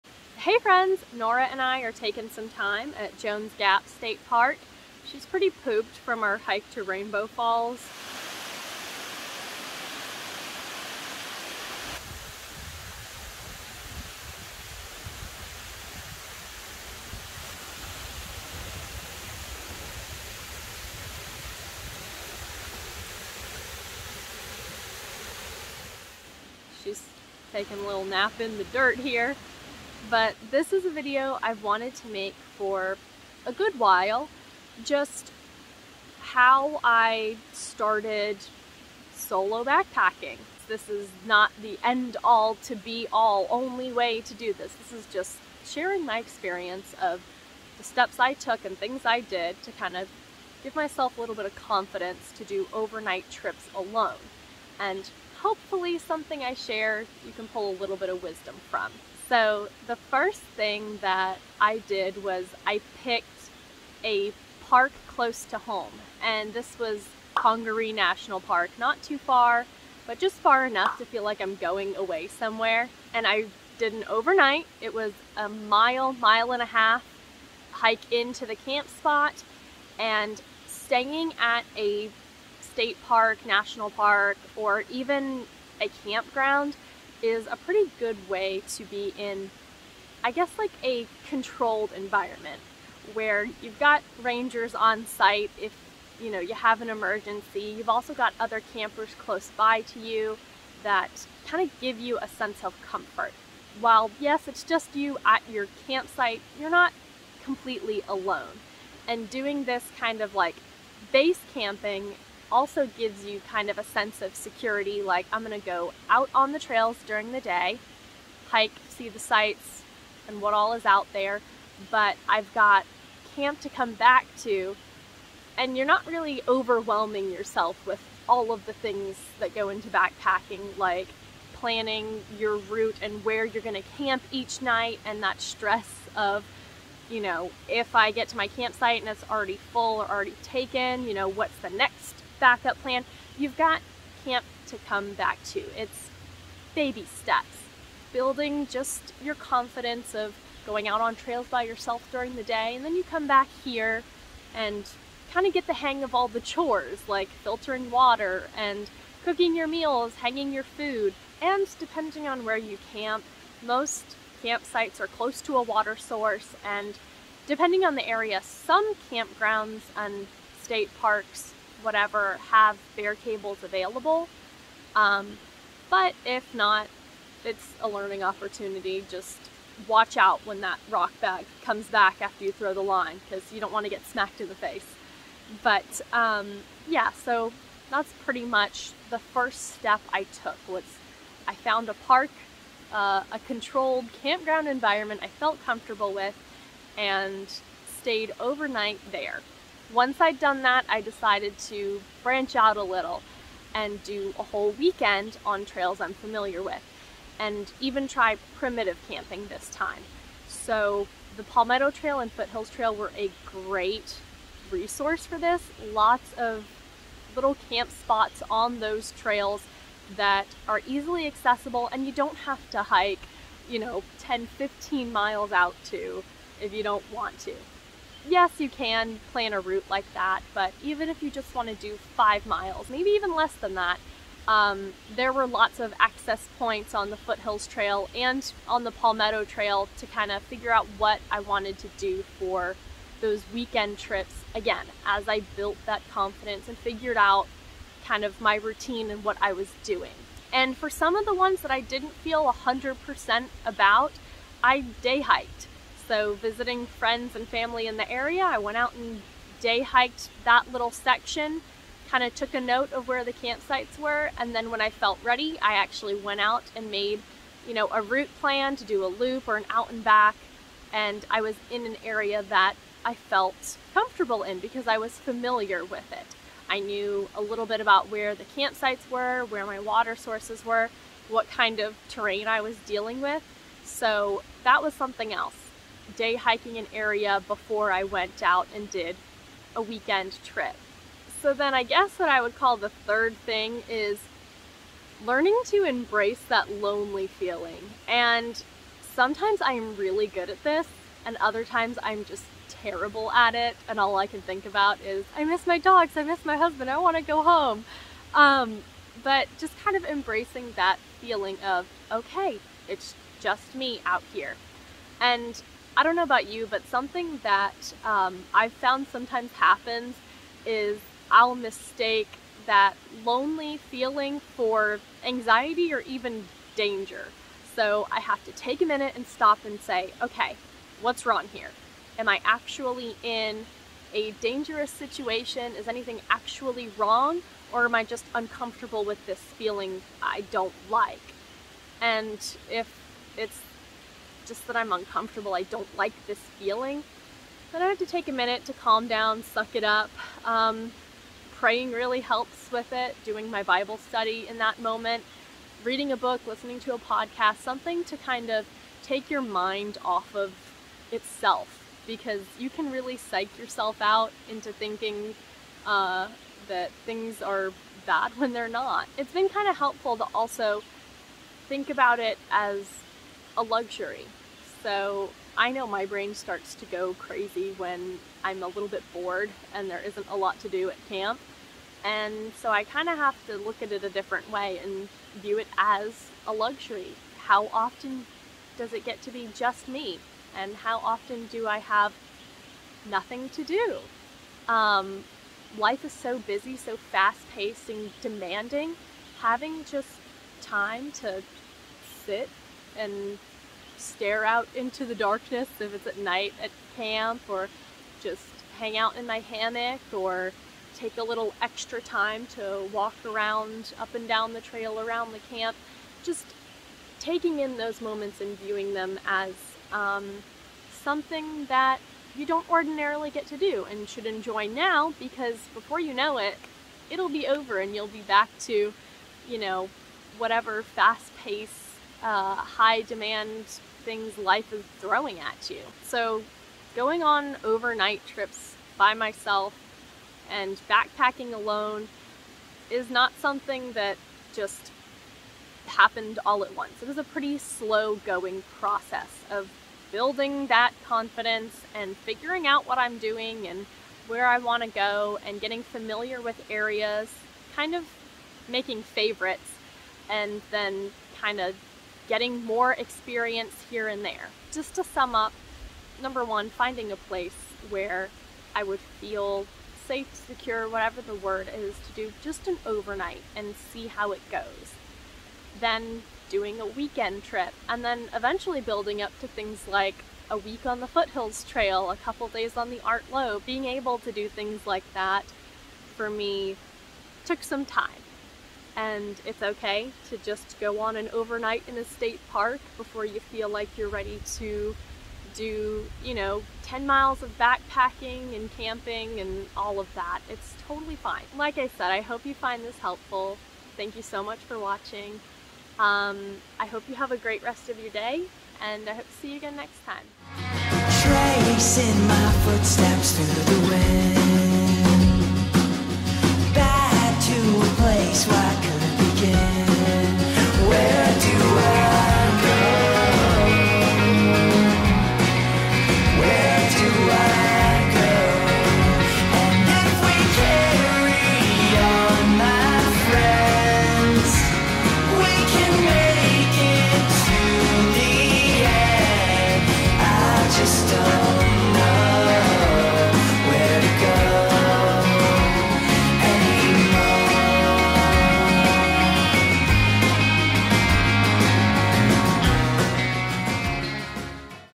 How To Start Backpacking Solo
Be sure to check out my blog post on this topic where I share a little more advice for how to start backpacking solo along with working through feelings of loneliness which can often ruin an otherwise great trip

Backpacking solo has been an incredibly fun and rewarding experience, but it definitely took some time before I felt comfortable and confident enough to take on multi-day trips. Today I'd like to share the 3 steps I took to get used to hiking, camping, and backpacking alone. Not ready to get out there alone just yet? That's totally fine! These tips can apply to backpacking with a partner or even small group too.

- Check out some of my downloadable trip itineraries

Before you go, don't forget to check out the videos and trail journals from some of my favorite solo trips thus far: